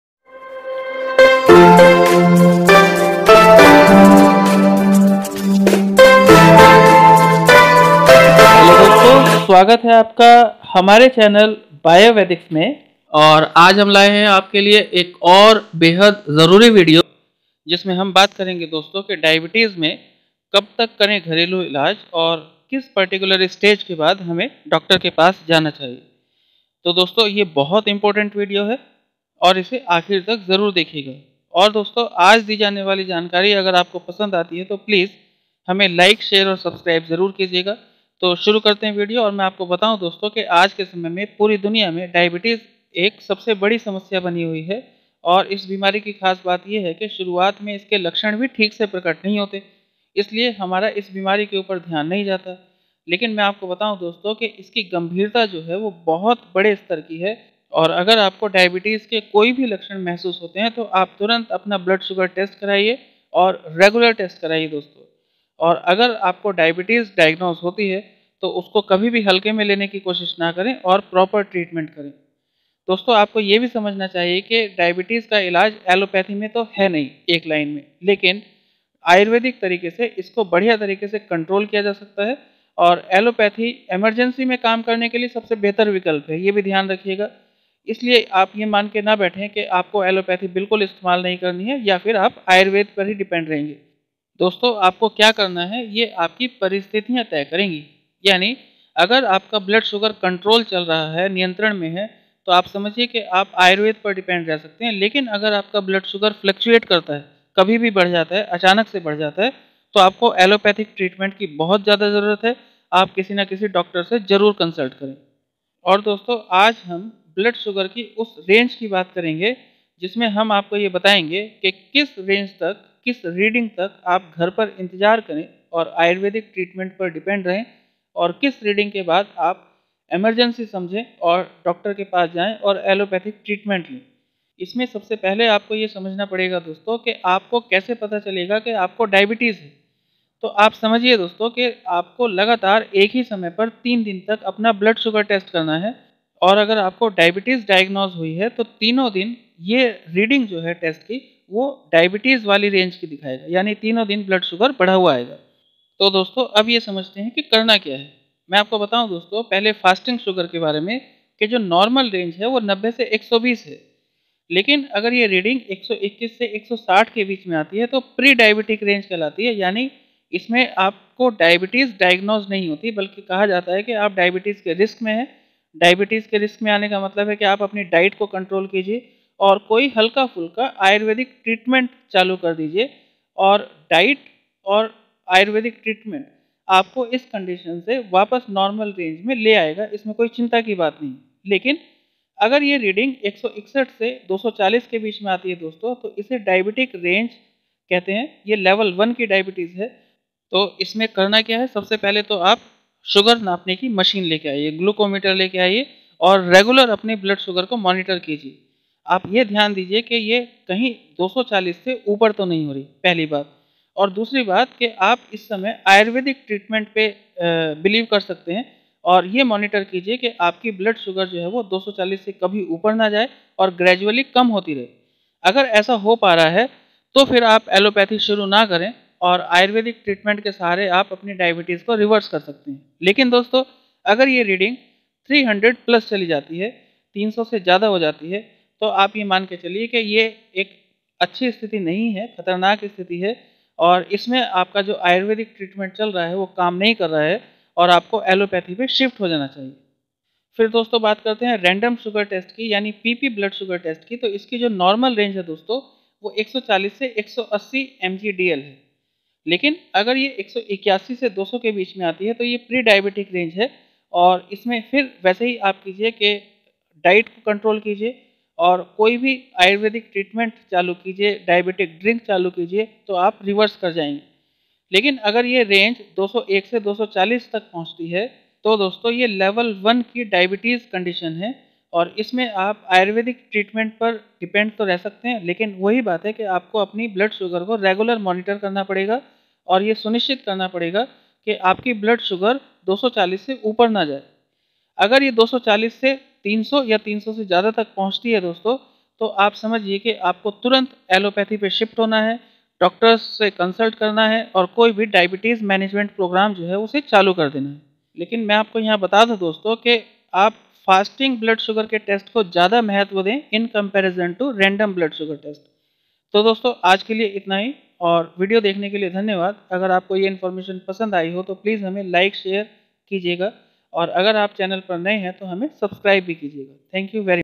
डायबिटीज | शुगर में कब तक करें घरेलू इलाज व कब जाएं डॉक्टर के पास | शुगर लेवल चार्ट
when you should contact doctor for diabetes

 in this video I explain about the conditions and risk factors of diabetes by which we can decide when to contact the doctor and when we should treat diabetes at home

 risk level of diabetes
 pre diabetic condition

diabetic condition

diabetes risk level 1

Diabetes risk level 2

symptoms of diabetes

किस आधार पर और किन परिस्थितियों में यह तय करें कि आपको डायबिटीज का इलाज घर पर रहकर करना है या फिर डॉक्टर से संपर्क करना है

डायबिटीज का एलोपैथी में कोई सीधा इलाज तो है नहीं लेकिन फिर भी डायबिटीज के मरीजों को किसी भी इमरजेंसी से निपटने के लिए एलोपैथी का सहारा जरूर लेना चाहिए

आज के वीडियो में अभी अभी जाने के लिए डायबिटीज में ब्लड शुगर लेवल की रेंज कितनी होनी चाहिए यानी नॉरमल फास्टिंग शुगर लेवल कितना होना चाहिए और नॉर्मल रैंडम शुगर लेवल कितना होना चाहिए

 डायबिटीज में प्रीडायबीटिक और डायबिटिक रेंज क्या होती है इसके बारे में भी आज के वीडियो में हम आपको बताएंगे

डायबिटीज को पहचानने के लिए किस टेस्ट पर ज्यादा विश्वास करना चाहिए

डायबिटीज टेस्ट के द्वारा कैसे पहचाने कि हमें डायबिटीज है या नहीं